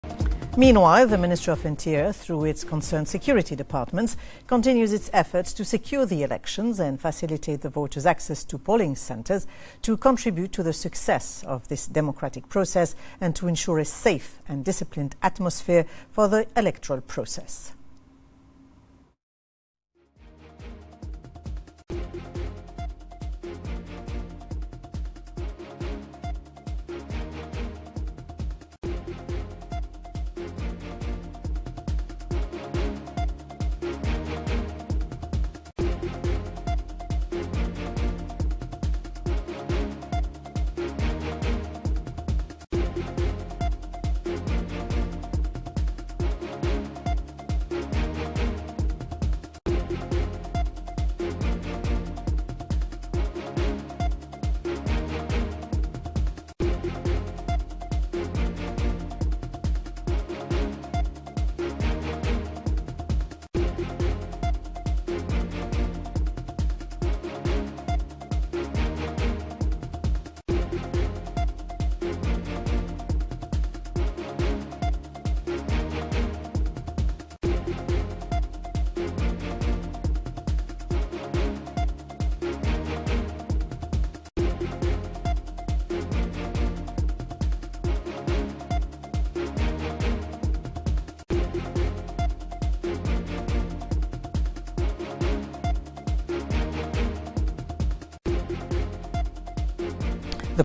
Interior Ministry continues efforts to secure, facilitate electoral process
رابط الخاص بحساب التويتر